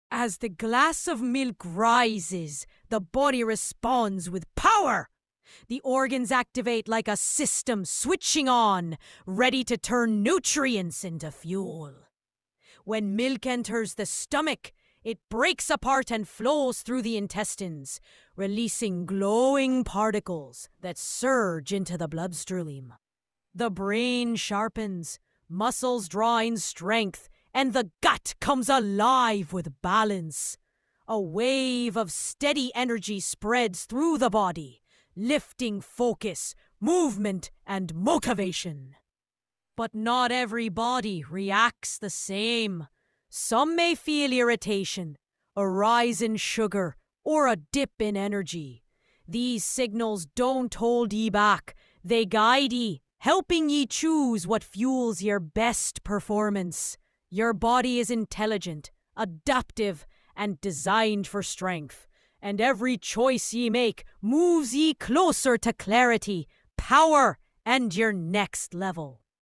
How Your Body Digests Milk in 3D | Amazing Human Digestive System Animation
Discover what really happens inside your body when you drink milk! 🥛
This short 3D animation shows how milk travels through your mouth, stomach, and intestines — breaking down into nutrients your body uses for energy, growth, and health.

Perfect for students, science lovers, and anyone curious about the human body.
Watch till the end for the full digestion process!

👉 Topics Covered:
• How milk enters the digestive system
• Stomach acid + enzyme breakdown
• Nutrient absorption in the small intestine
• 3D realistic digestive animation
• Quick and easy science explanation

📌 Follow for more 3D science animations!